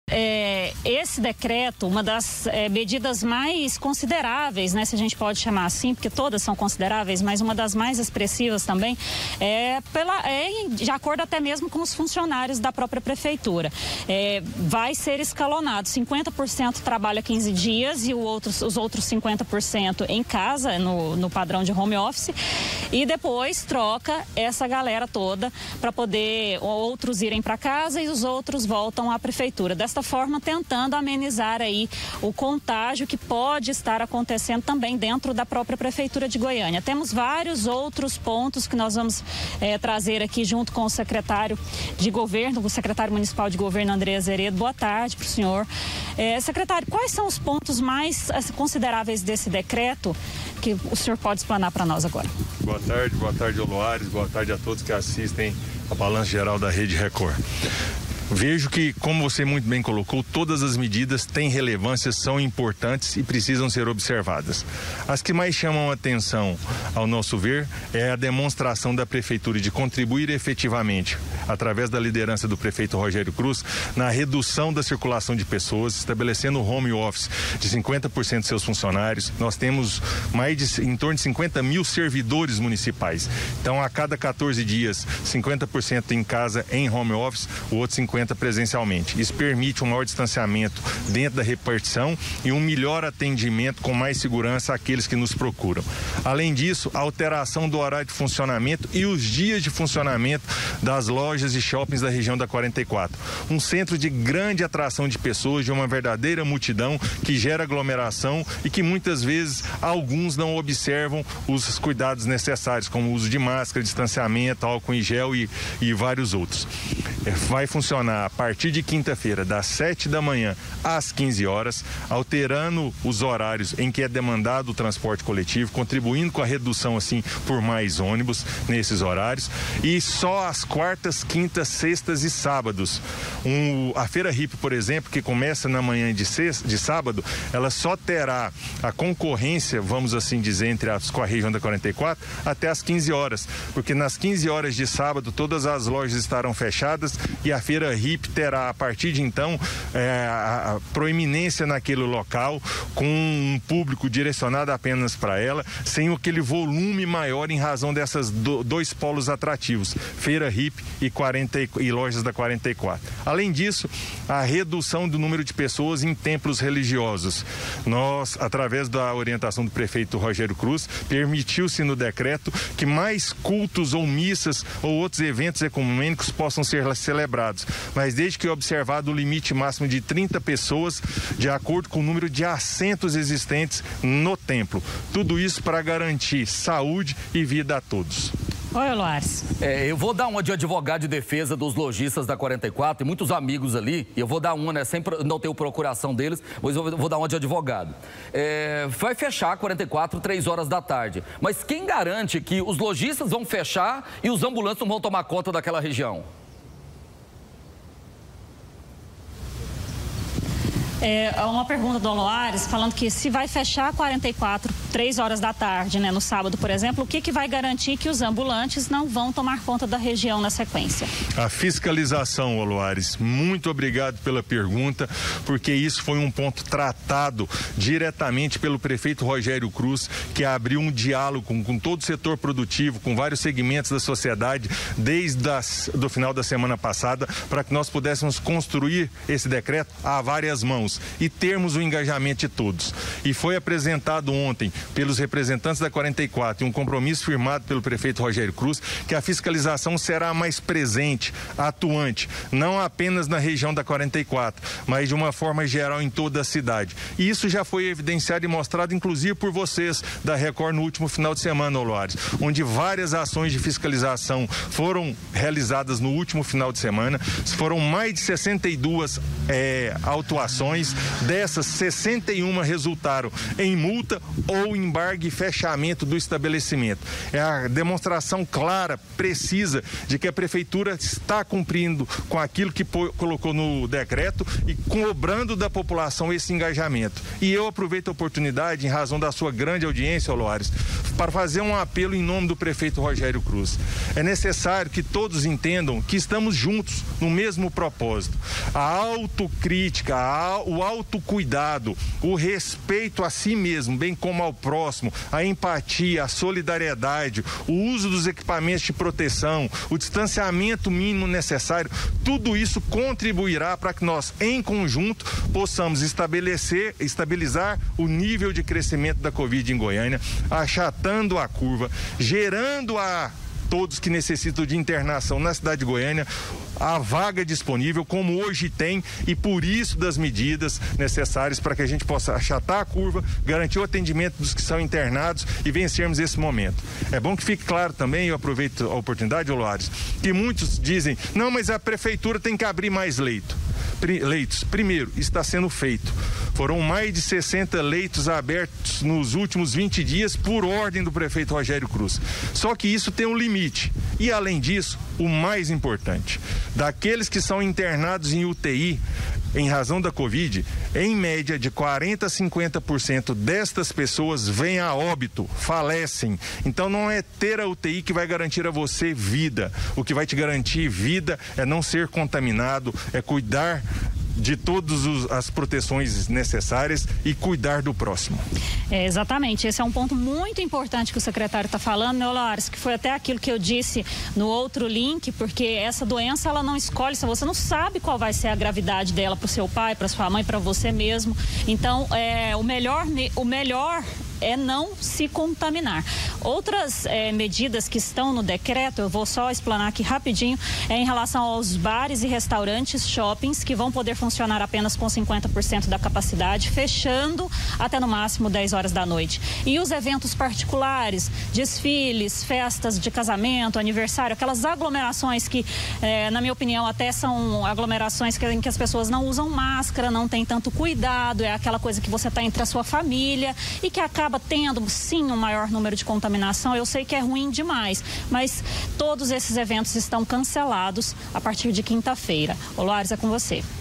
NOVO DECRETO: NOVAS RESTRIÇÕES PARA CONTER A COVID-19
AS NOVAS RESTRIÇÕES CONTRA O AVANÇO DA COVID-19 EM GOIÂNIA ENTRAM EM VIGOR NA QUINTA-FEIRA.